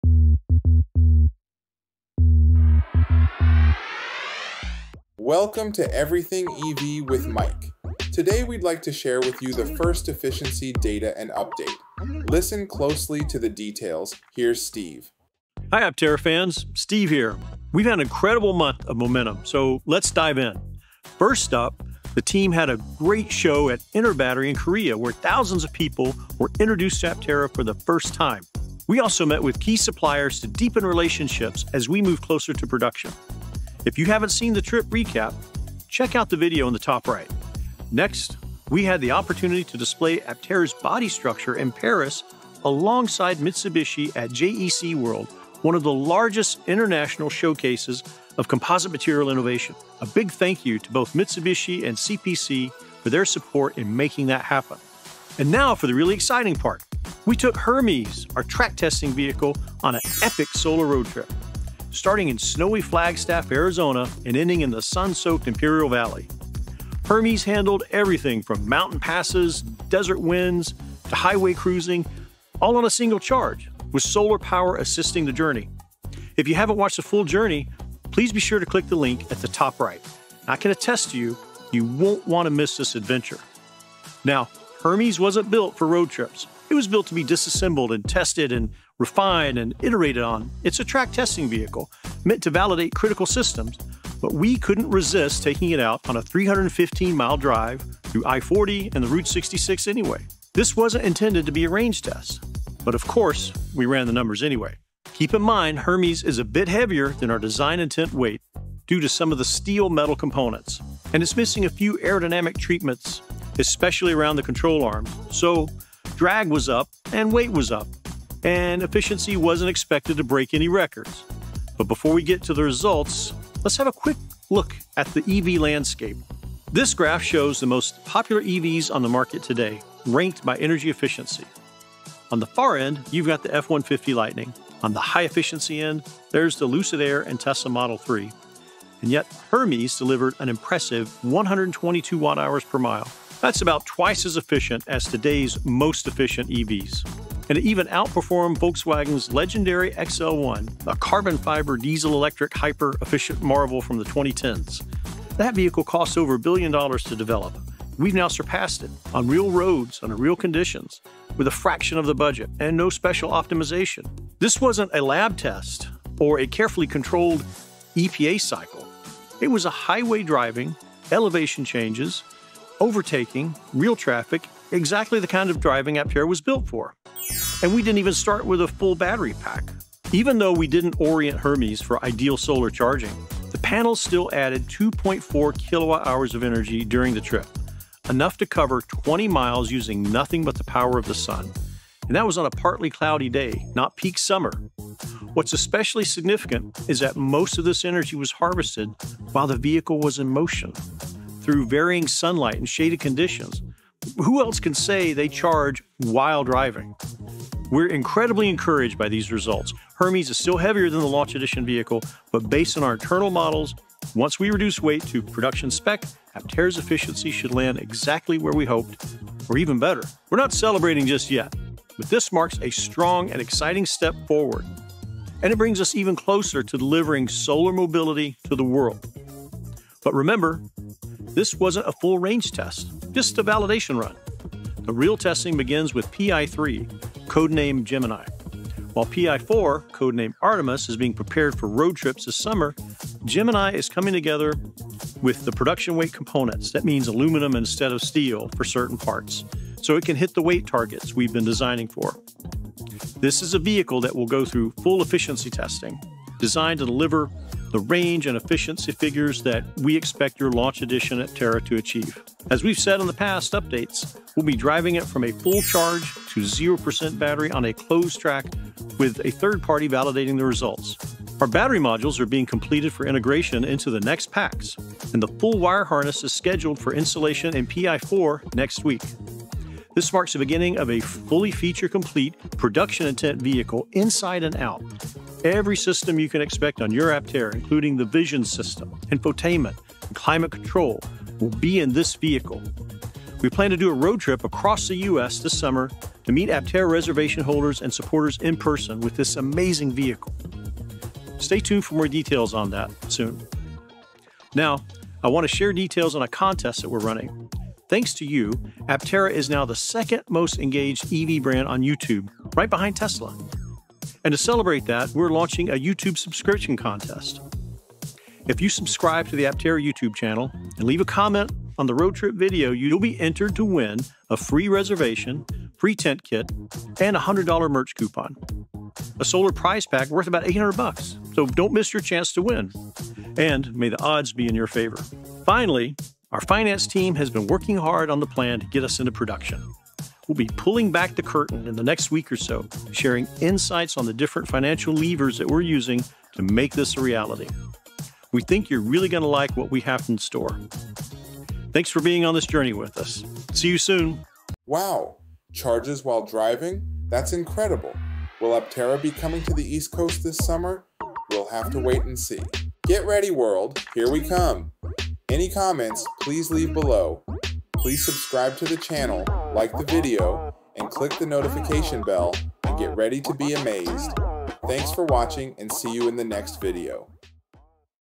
⚡️Breaking Down Aptera’s Latest Efficiency Numbers & What’s Next⚡️Aptera
Aptera Motors has just completed its first-ever road trip with their production-intent solar electric vehicle, and the efficiency data is turning heads across the EV world. Covering over 300 miles on a single charge—including snowy mountain passes, historic Route 66, and desert highways—the Aptera achieved a remarkable 122 Wh/mile, making it twice as efficient as the leading electric vehicles on the market today.
During the journey, Aptera harvested 2.4 kWh of solar energy, translating to 20 miles of driving powered solely by the sun—even under partly cloudy skies. This road trip not only demonstrates Aptera’s industry-leading efficiency and solar charging capabilities but also marks a major milestone toward sustainable, long-range, zero-emission travel.